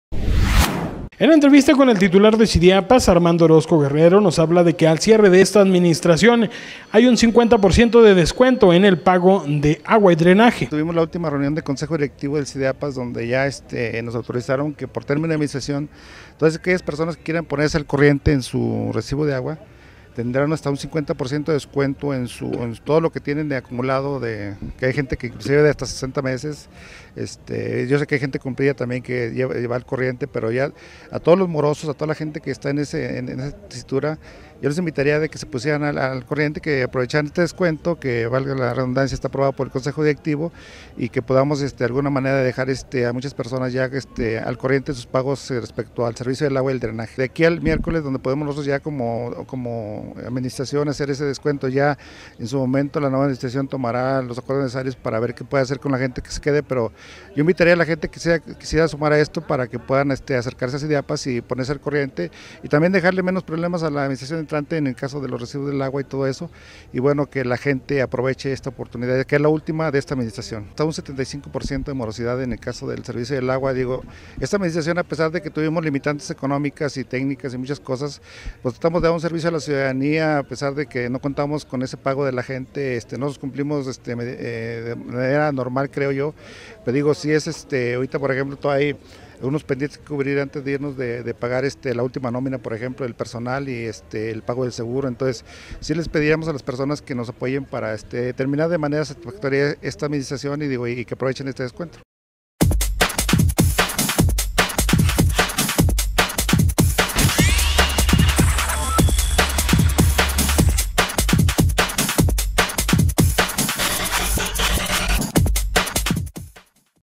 El Salto l Ofrecen descuentos en Sideapas de hasta el 50 por ciento
Pueblo Nuevo, DGO.- Estos últimos días que queda de administración municipal en el Sistema Descentralizado de Agua Potable Alcantarillado y Saneamiento (Sideapas) habrá descuentos de hasta el 50 por ciento, informó Armando Orozco titular de mencionada dependencia.